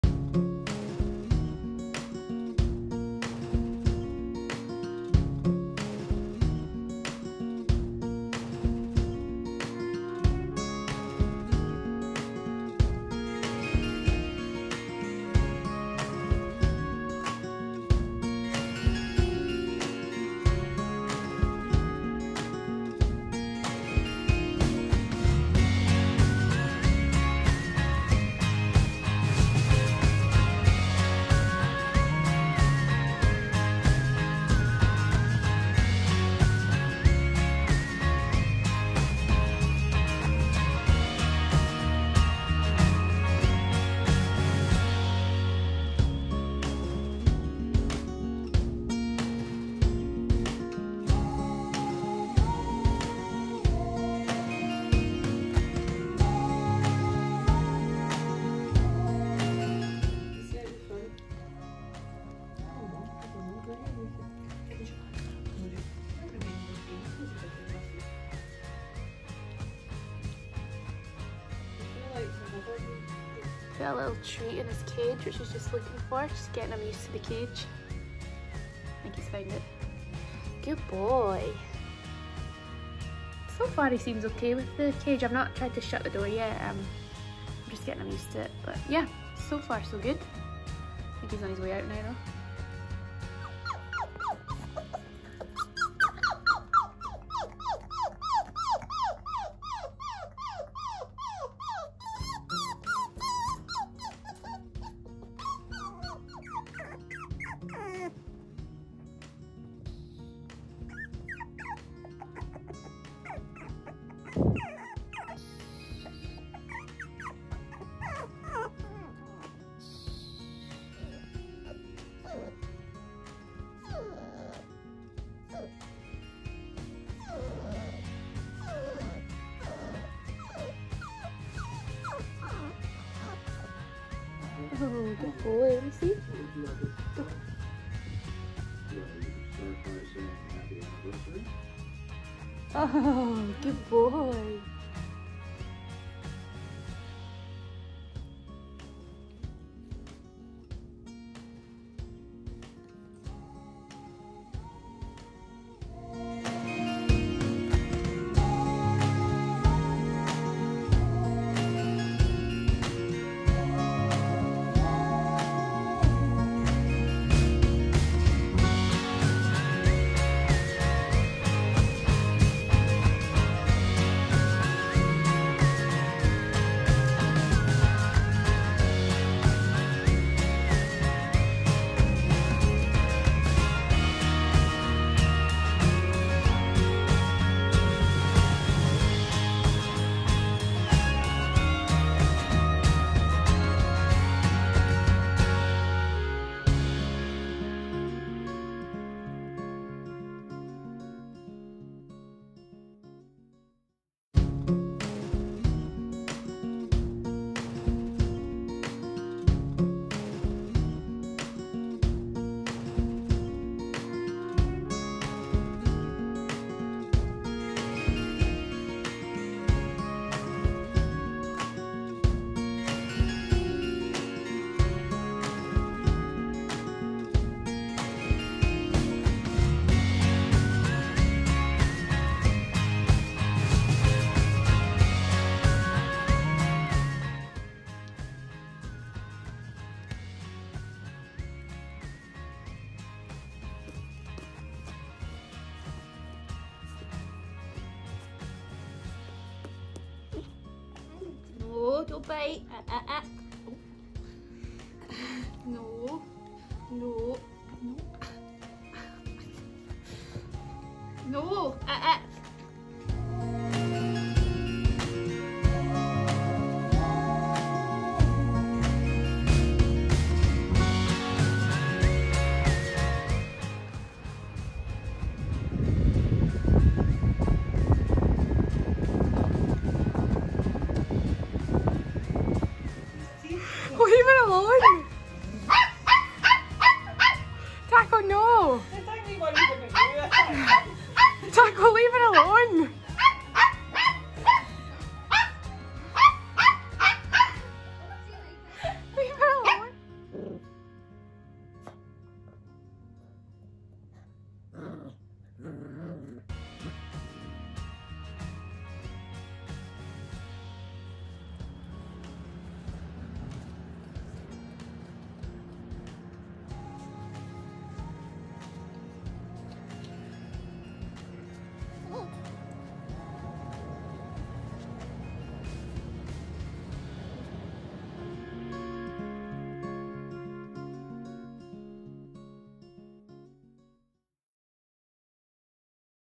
First week home with Cockapoo puppy
Our first day and week home with our 8 week old cockapoo puppy, Taco.

Link to dog bed